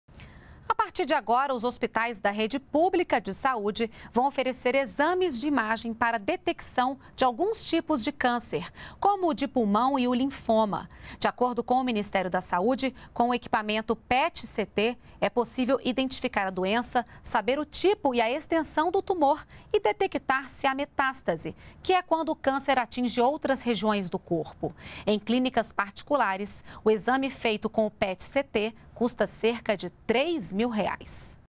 SUS vai oferecer exames de imagem para detecção de alguns tipos de câncer
NBR NOTÍCIAS -- 23.04.14: A partir de agora, os hospitais da rede pública de saúde vão oferecer exames de imagem para detecção de alguns tipos de câncer, como o de pulmão e o linfoma. De acordo com o Ministério da Saúde, com o equipamento PET-CT é possível identificar a doença, saber o tipo e a extensão do tumor e detectar se há metástase, que é quando o câncer atinge outras regiões do corpo. Em clínicas particulares, o exame feito com o PET-CT custa cerca de R$ 3 mil.